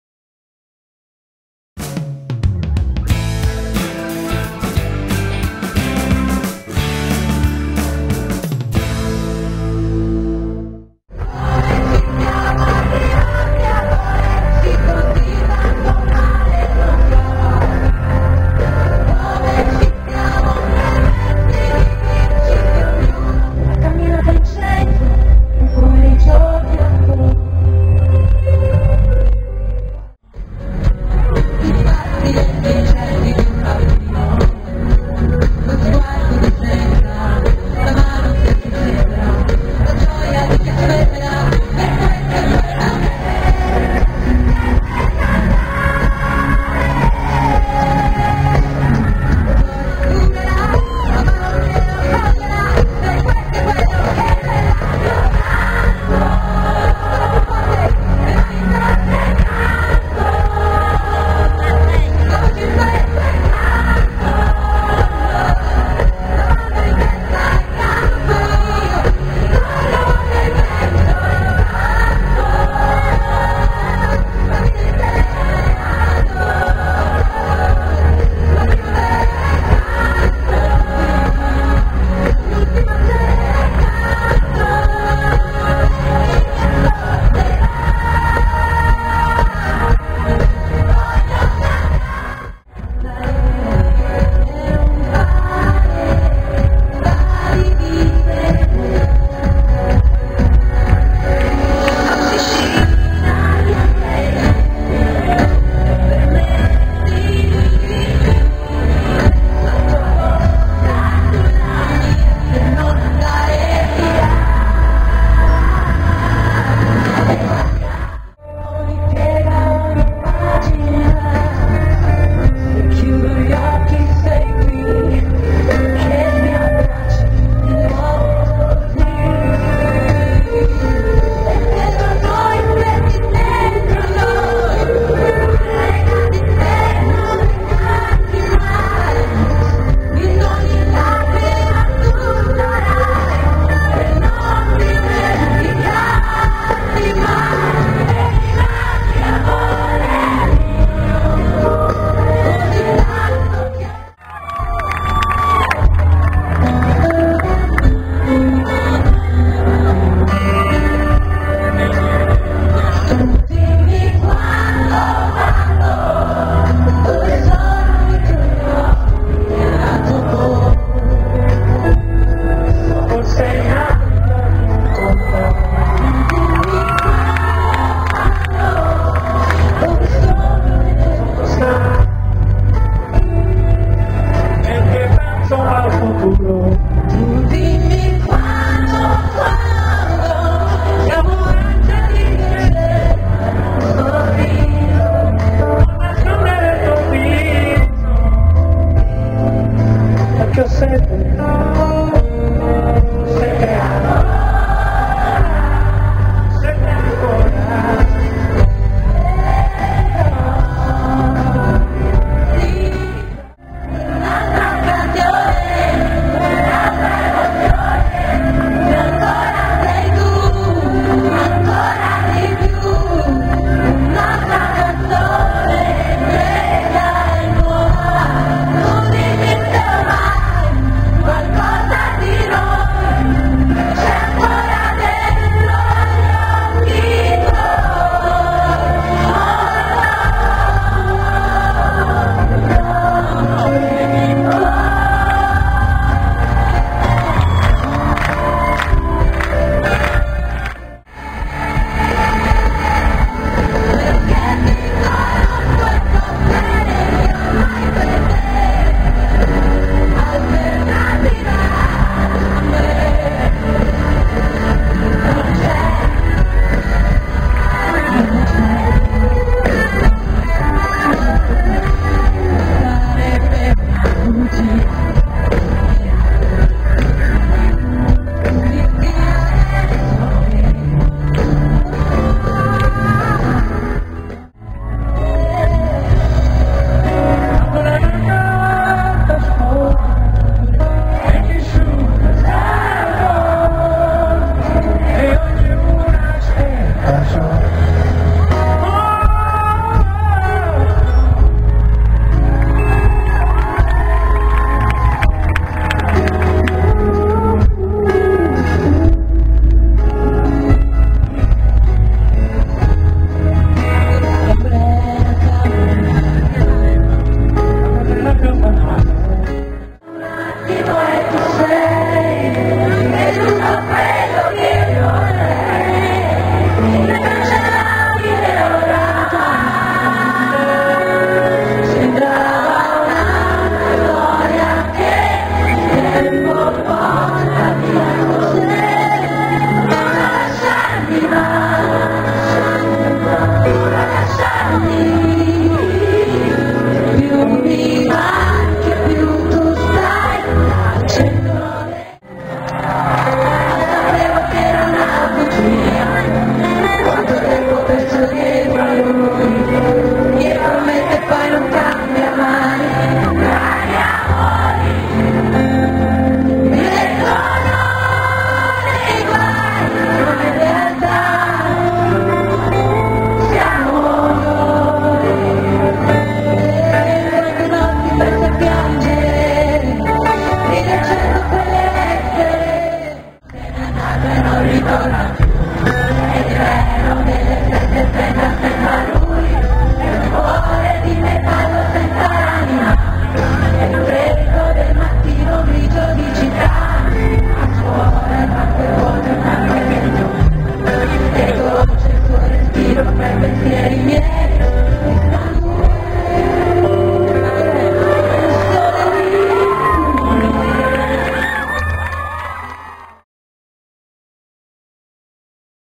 Laura Pausini - concerto Napoli Piazza del Plebiscito - 24 luglio 2012
Tre anni di assenza da Napoli. È dunque più che mai atteso il ritorno di Laura Pausini in città. Il prossimo 24 luglio l'Inedito World Tour approderà a Piazza del Plebiscito, luogo simbolo riservato solo ai grandi protagonisti della musica.

"Suonare in Piazza Plebiscito rappresenta per me il realizzarsi di un sogno. Napoli è infatti città di lunga tradizione musicale. Ringrazio il sindaco e l'amministrazione per avermi dato questa possibilità" ha dichiarato la cantante.

L'evento esclusivo è stato possibile, infatti, grazie all'intervento del sindaco Luigi De Magistris e dell'amministrazione comunale di Napoli che hanno fortemente voluto e creduto nel progetto."